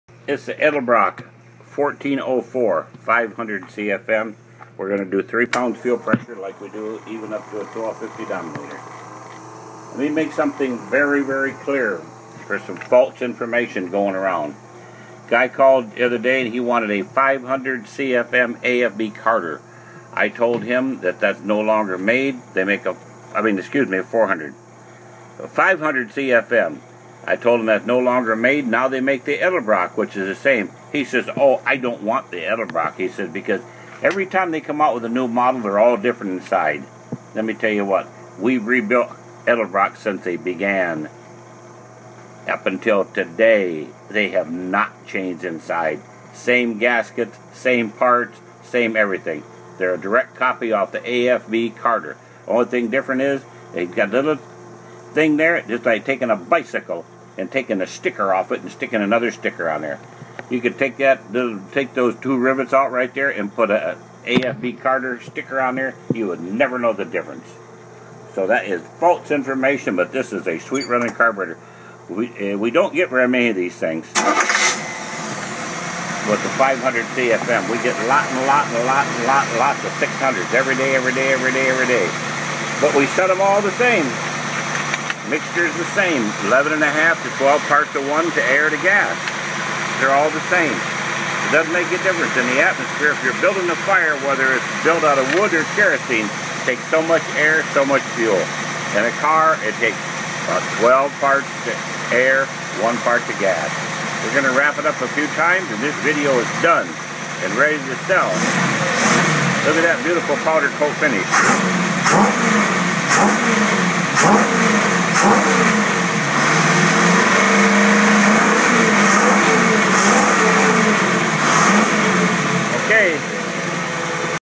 Edelbrock 1404 carb
This is an edelbrock 1404 carb that is being tested on a running engine.